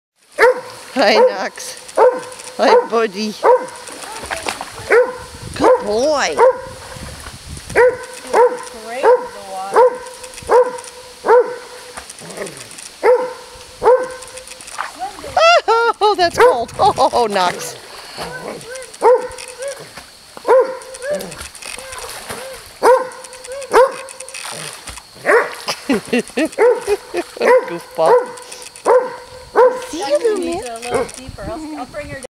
Self entertaining Doberman
Dazey is perfectly capable of enjoying a swim and some games all by herself.  (Please excuse the squealing from the photographer whose puppy was splashing around in the cold river water.)